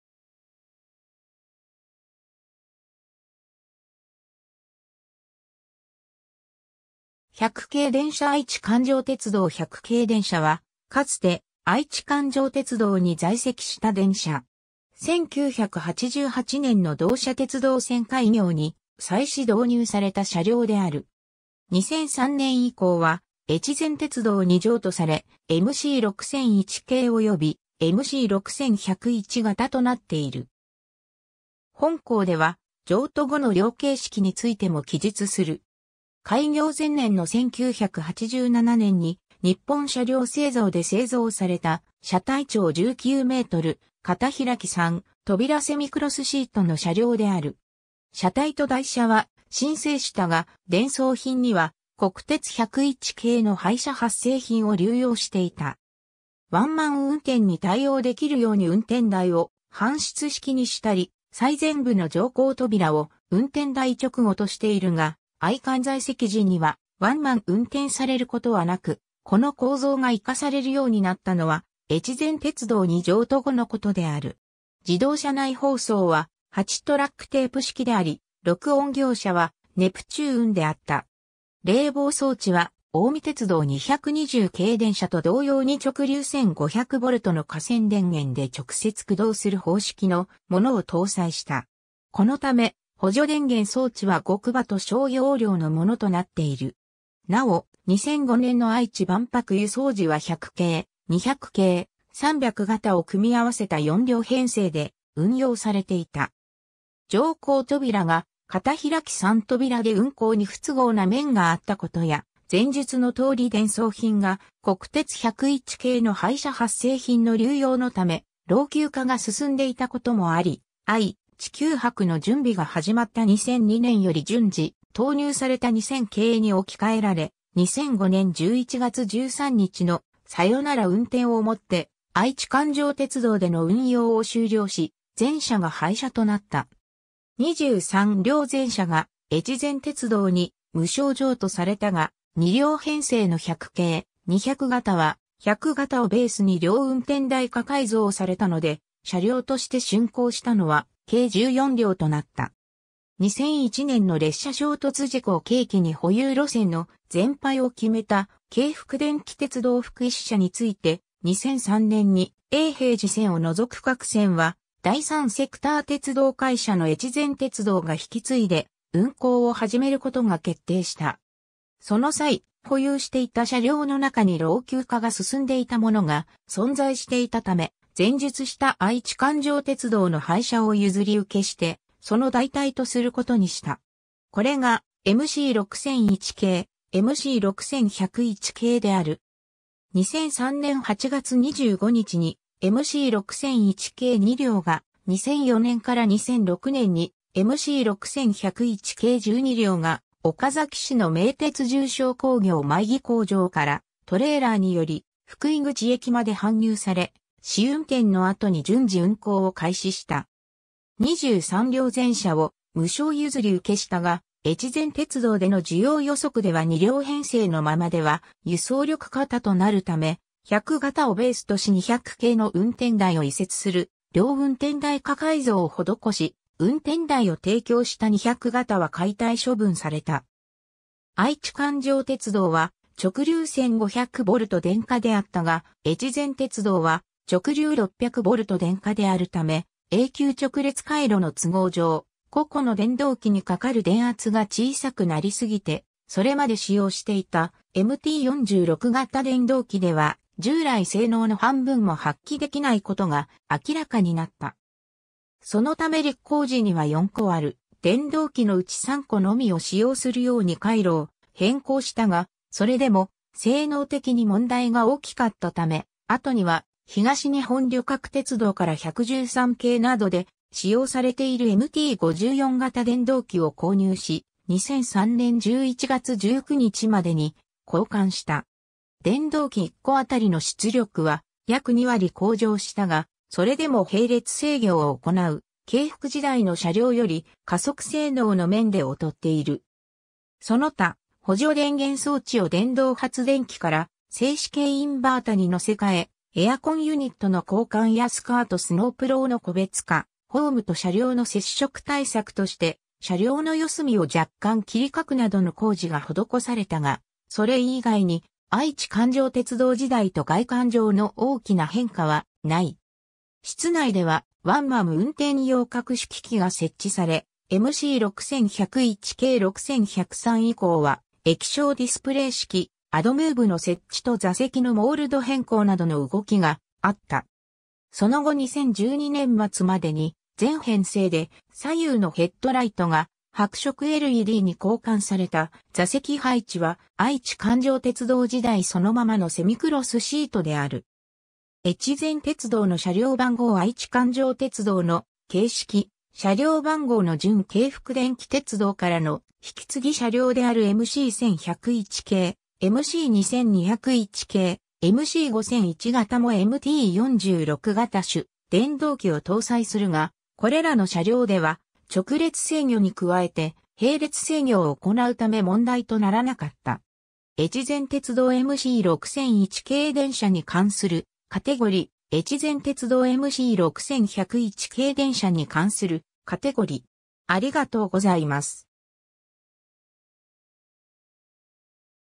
愛知環状鉄道100系電車
100系電車 （2005年3月7日 万博八草駅） 愛知環状鉄道100系電車（あいちかんじょうてつどう100けいでんしゃ）は、かつて愛知環状鉄道に在籍した電車。
1988年（昭和63年）の同社鉄道線開業に際し導入された車両である。
 2003年（平成15年）以降はえちぜん鉄道に譲渡され、MC6001形（エムシー6001がた）およびMC6101形（エムシー6101がた）となっている。
本項では、譲渡後の両形式についても記述する。
 開業前年の1987年（昭和62年）に日本車輌製造で製造された車体長19m、片開き3扉セミクロスシートの車両である。
車体と台車は新製したが、電装品には国鉄101系の廃車発生品を流用していた。
 ワンマン運転に対応できるように運転台を半室式にしたり、最前部の乗降扉を運転台直後としているが、愛環在籍時にはワンマン運転されることはなく、この構造が活かされるようになったのはえちぜん鉄道に譲渡後のことである。
 自動車内放送は、8トラックテープ式であり、録音業者はネプチューン（現・レゾナント・システムズ）であった。
 冷房装置は、近江鉄道220形電車と同様に直流1500Vの架線電源で直接駆動する方式のものを搭載した。
このため補助電源装置は5kVAと小容量のものとなっている。
 なお、2005年（平成17年）の愛知万博（愛・地球博）輸送時は100形・200形・300形を組み合わせた4両編成で運用されていた。
 乗降扉が片開き3扉で運行に不都合な面があった事や、前述のとおり電装品が国鉄101系の廃車発生品の流用のため老朽化が進んでいた事もあり、愛・地球博の準備が始まった2002年より順次投入された2000系に置き換えられ、2005年11月13日のさよなら運転をもって愛知環状鉄道での運用を終了し、全車が廃車となった。
23両全車がえちぜん鉄道に無償譲渡されたが、2両編成の100形・200形は、100形をベースに両運転台化改造されたので、車両として竣工したのは計14両となった。
 2001年の列車衝突事故を契機に保有路線の全廃を決めた京福電気鉄道福井支社について、2003年に永平寺線を除く各線は第三セクター鉄道会社のえちぜん鉄道が引き継いで運行を始めることが決定した。
その際、保有していた車両のなかに老朽化が進んでいたものが存在していたため、前述した愛知環状鉄道の廃車を譲受してその代替とすることにした。
これがMC6001形・MC6101形である。
 2003年8月25日にMC6001形2両（6001・6002）が、2004年 - 2006年にMC6101形12両 (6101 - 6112) が岡崎市の名鉄住商工業舞木工場からトレーラーにより福井口駅まで搬入され、試運転の後に順次運行を開始した。
 23両全車を無償譲受したが、えちぜん鉄道での需要予測では2両編成のままでは輸送力過多となるため、100形をベースとし200形の運転台を移設する両運転台化改造を施し、運転台を提供した200形は解体処分された。
 愛知環状鉄道は直流1500V電化であったが、えちぜん鉄道は直流600V電化であるため、永久直列回路の都合上、個々の電動機にかかる電圧が小さくなりすぎてそれまで使用していたMT46型電動機では従来性能の半分も発揮できないことが明らかになった。
そのため力行時には4個ある電動機のうち3個のみを使用するように回路を変更したが、それでも性能的に問題が大きかったため、後には東日本旅客鉄道（JR東日本）から113系などで使用されているMT54型電動機を購入し、2003年11月19日までに交換した。
電動機1個あたりの出力は約2割向上（架線電圧600V、3個直列での出力53kW→64kW）したが、それでも並列制御を行う京福時代の車両より加速性能の面で劣っている。
 その他、補助電源装置を電動発電機 (MG) から静止形インバータ (SIV) に載せ替え、エアコンユニットの交換やスカート・スノープロウの個別化（いずれもMC6101形）、ホームと車両の接触対策として車両の四隅を若干切り欠くなどの工事が施されたが、それ以外に愛知環状鉄道時代と外観上の大きな変化はない。
室内ではワンマン運転用各種機器が設置され、MC6101形6103以降は液晶ディスプレイ式アドムーブの設置と座席のモールド変更などの動きがあった。
 その後2012年末までに、全編成で左右のヘッドライトが白色LEDに交換された(...